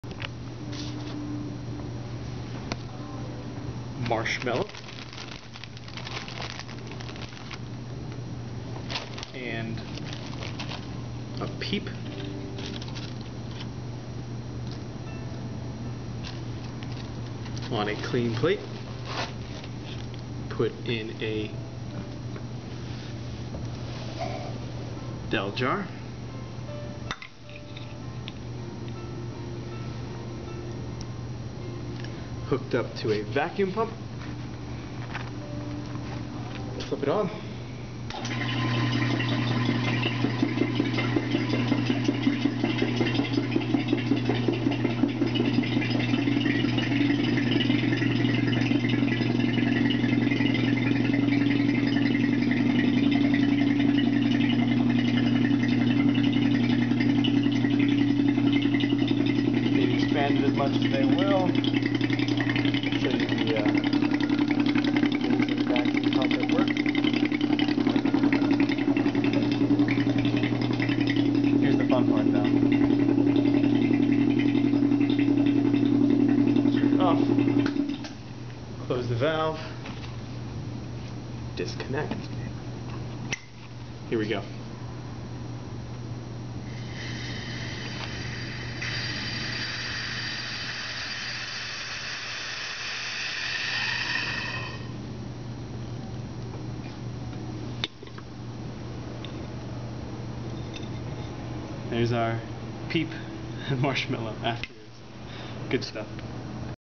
Peep and Marshmallow in a Vacuum Pump
In my physics class today, we were using a bell jar and a vacuum pump to further demonstrate the combined gas law.

As the vacuum pump removes the air from the bell jar, the marshmallow expands.  This is basically the reverse of blowing up a balloon... instead of increasing the pressure inside, we're decreasing the pressure outside.  Both processes increase the pressure differential.  When the marshmallow gets large enough, there's a small breach in the surface, which allows the pressure in the marshmallow to equalize with the pressure outside.

When the air re-enters the bell jar, the inside of the marshmallow had been at a lower pressure and the volume decreases as if that were its original pressure... leaving the marshmallow void of most of the air that was once inside of it.

TL:DR - Peep + Vacuum Pump = Deflated Peep